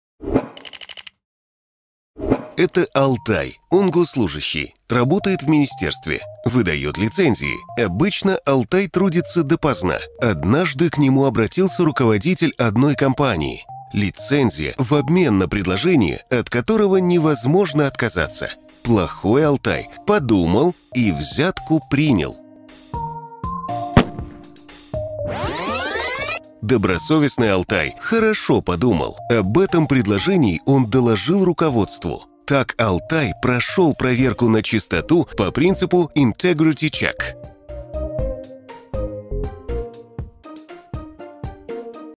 antikor-2
Агенттік Қазақстан Республикасының сыбайлас жемқорлыққа қарсы саясатының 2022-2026 жылдарға арналған тұжырымдамасы шеңберінде көзделген қаржылық тергеу, «integrity check» институты және мемлекеттік қызметшілердің шығыстары мен олардың кірістерімен сәйкестігін тексеру бастамалары бойынша түсіндірме бейнероликтерді әзірледі.